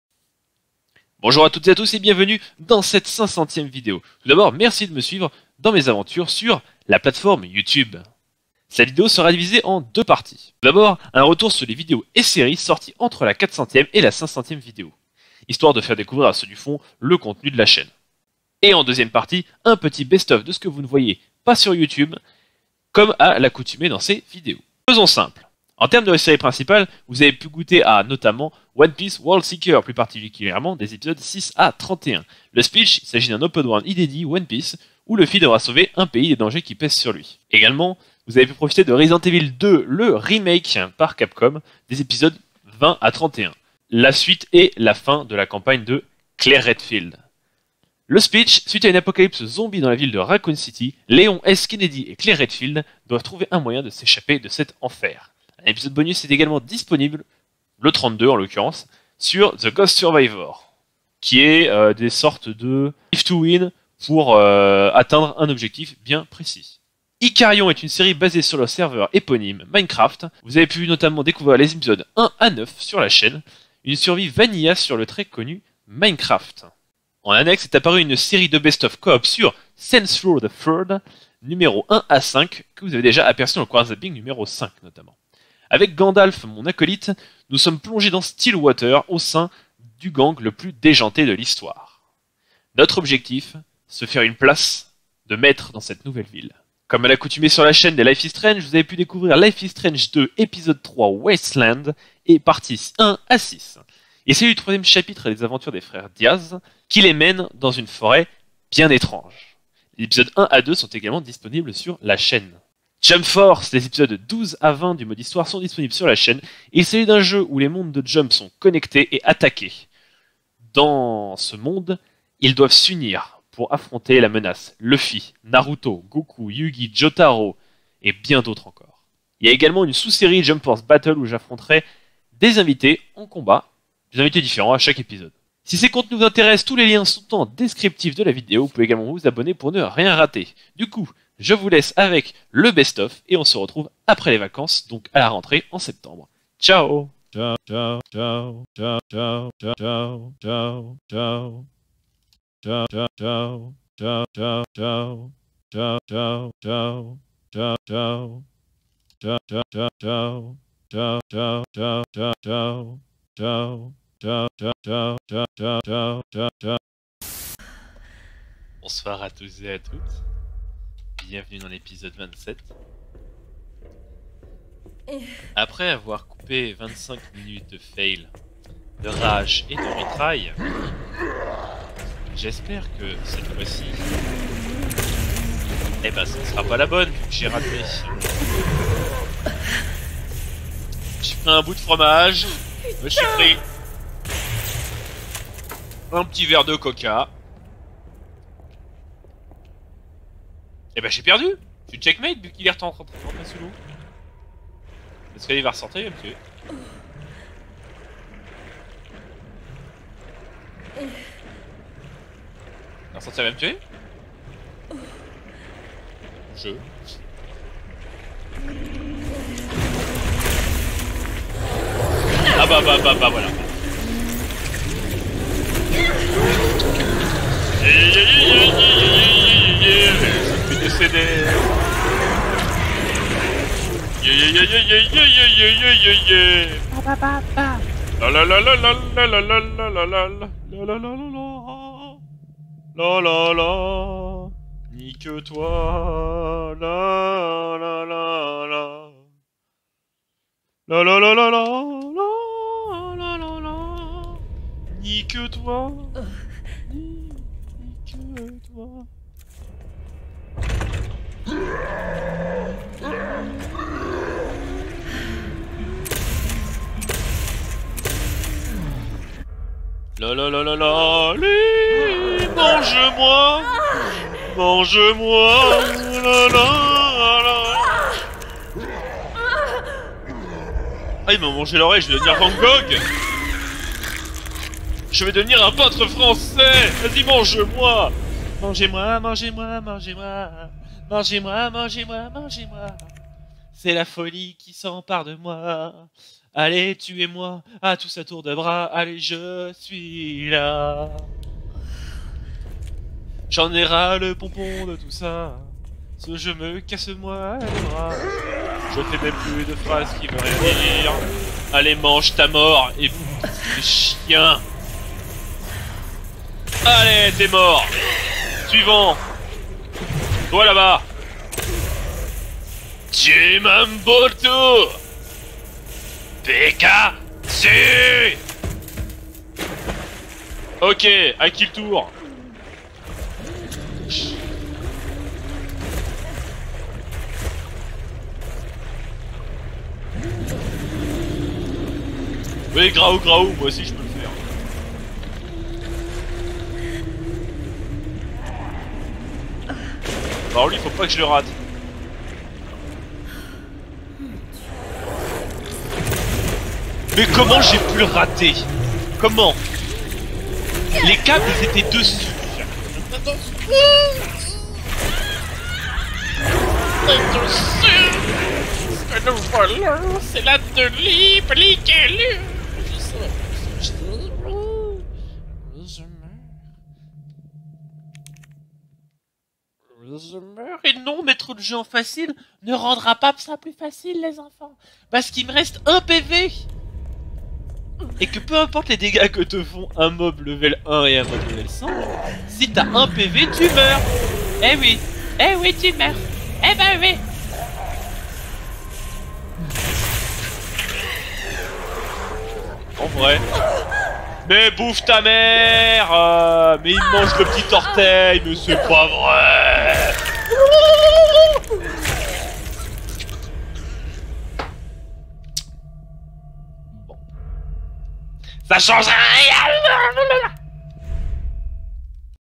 Présentation Chaîne Quartzall - Spécial 500ème Vidéo.
500ème vidéo ! Merci d'être encore là à suivre mes folles aventures !
Et bien sur un magnifique Best Of ne contenant aucun spoiler sur les séries en cours !